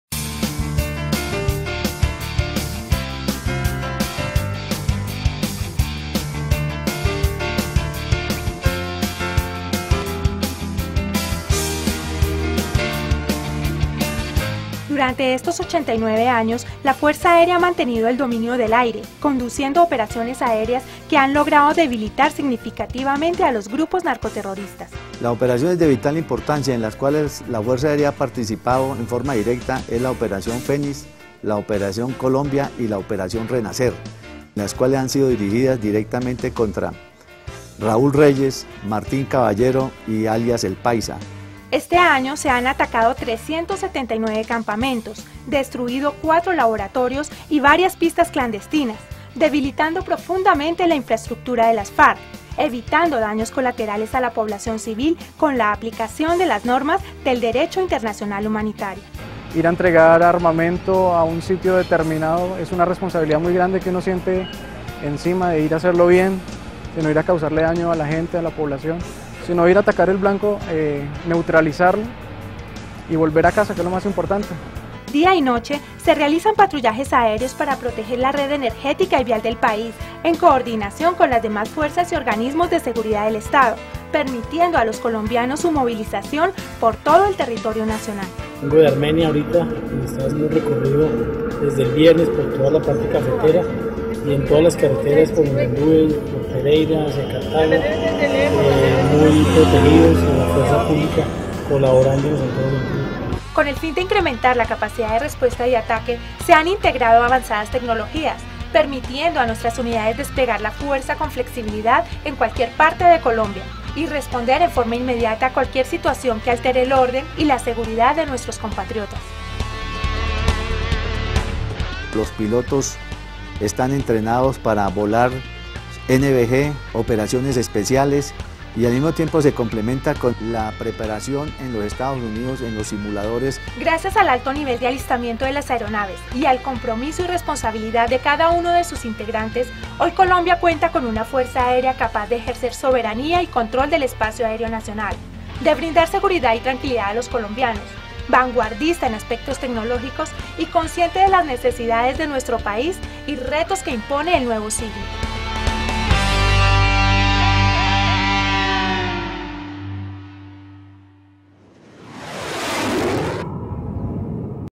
CSyD_FAC_90_Aniv.mp4
Video Institucional de la Fuerza Aérea Colombiana
90 Aniversario de la FAC